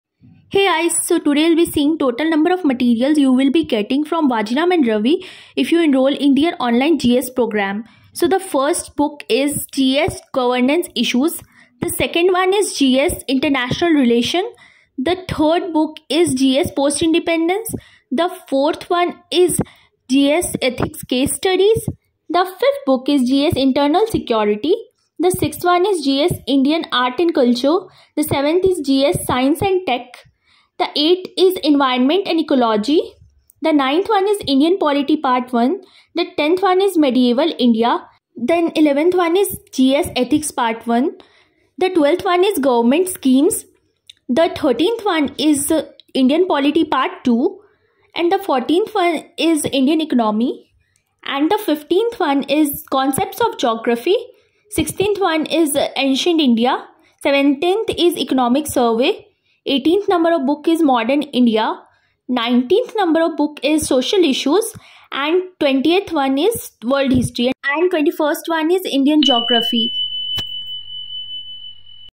Number of yellow books provided by vajiram for online GS class| Vajiram and Ravi yellow books
In this video I have shared total number of yellow books you will be receiving if you enroll in vajiram online GS classes .

regards,
girl writing destiny